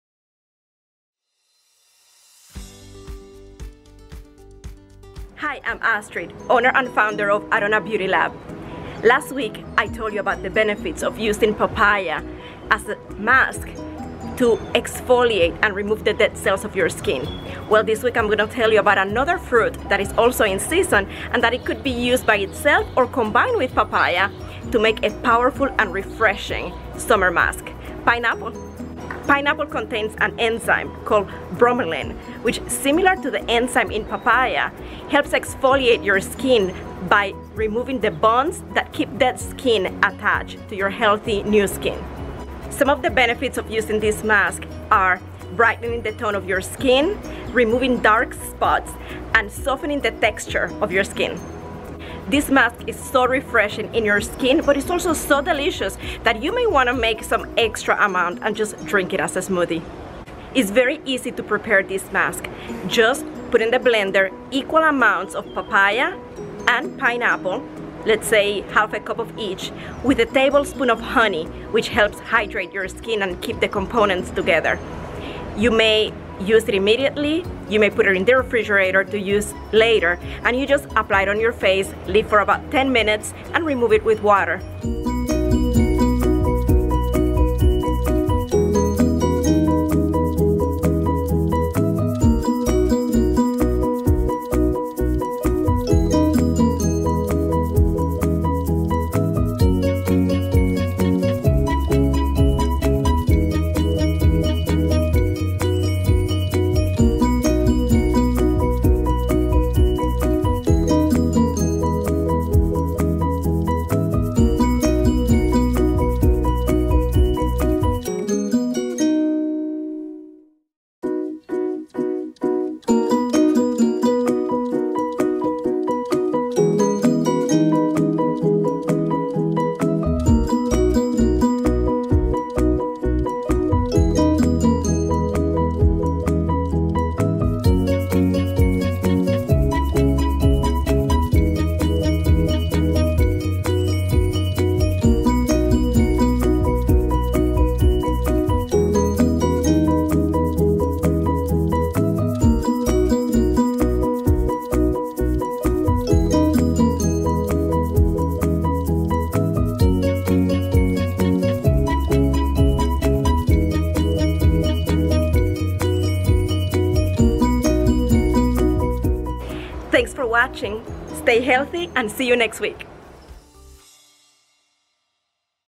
Benefits of Pineapple (Skin Care)
Pineapple contains an enzyme called Bromelain, which similar to papain, breaks down bonds that hold dead cells on top your new cells. 💁🏻‍♀️
The pineapple mask helps reducing the appearance of skin brown spots, brighten the skin tone, and soften the texture of your skin. ✋🏻
So enjoy pineapples which are in season and add them to your BBQ desert and to your smoothies too! 😋